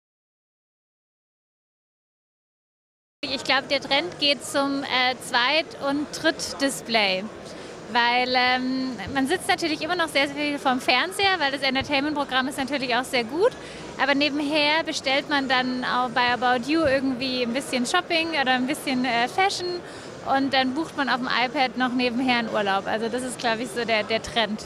APA at 4Gamechangers - Medienkonsum der Zukunft
Wie wird Ihr Medienkonsum in Zukunft aussehen? Wir haben dazu ein paar Besucherinnen und Besucher am  4Gamechangers-Festival befragt.